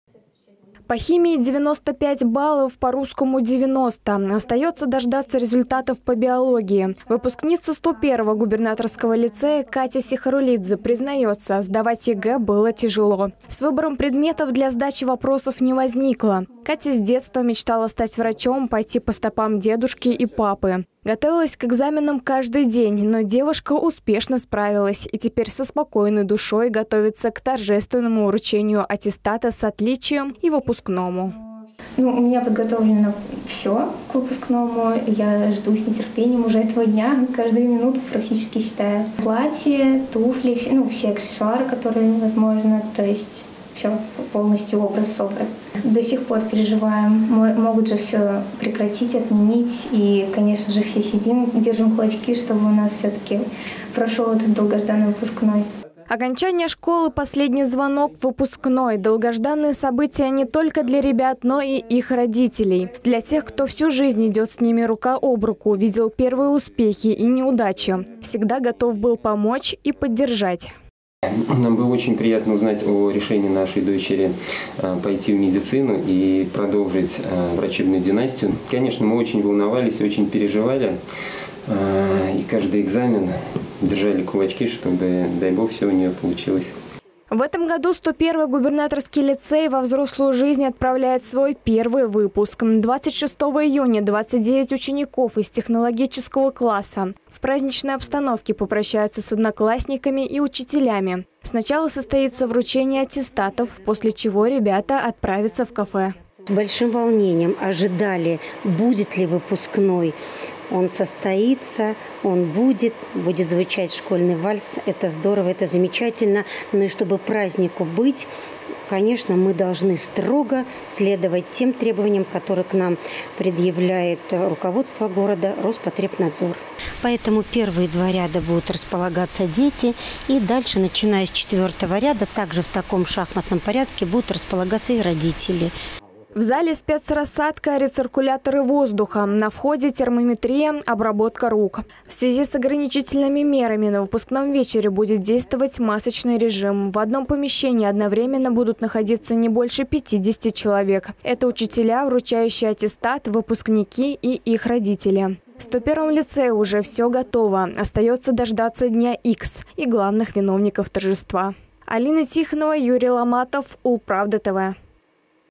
Праздник с ограничениями. Как пройдут выпускные в Ульяновске
Торжественный банкет, вечерние наряды и встреча рассвета. Всего этого выпускников лишила нахлынувшая пандемия в 2020-м. В этом году ребятам повезло чуть больше. 25, 26 и 27 июня по всей Ульяновской области для школьников всё-таки пройдут выпускные вечера в очном формате, но с соблюдением мер безопасности. Подробнее в нашем сюжете.